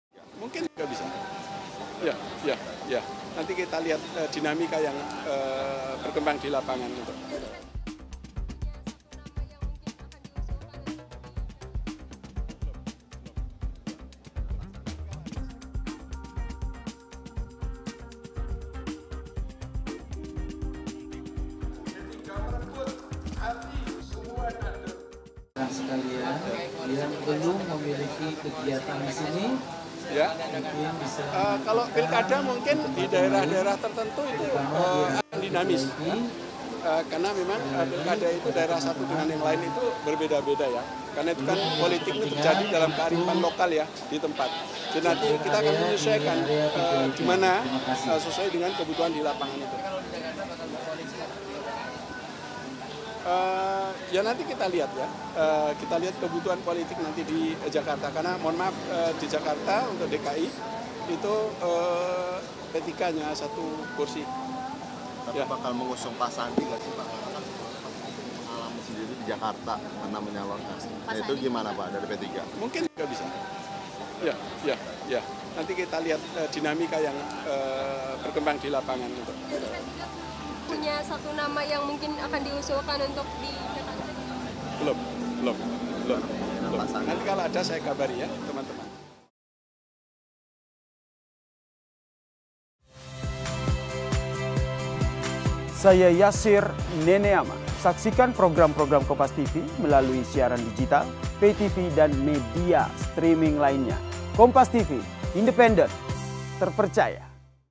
PPP Pertimbangkan Nama Sandiaga Uno untuk Pilgub DKI Jakarta
JAKARTA, KOMPAS.TV - Plt Ketua Umum PPP, Mardiono buka suara terkait kemungkinan partainya mengusung Sandiaga Uno dalam Pilkada Jakarta 2024.

"Ya nanti kita lihat ya, kita lihat kebutuhan politik di Jakarta. Karena mohon maaf, di Jakarta untuk DKI itu PPP nya satu kursi," ujar Plt Ketum PPP, Mardiono pada Sabtu, (28/4/2024).

"Mungkin juga bisa, nanti kita lihat dinamika yang berkembang di lapangan," jawab Mardiono saat ditanya kemungkinan mengusung Sandi di Pilkada Jakarta.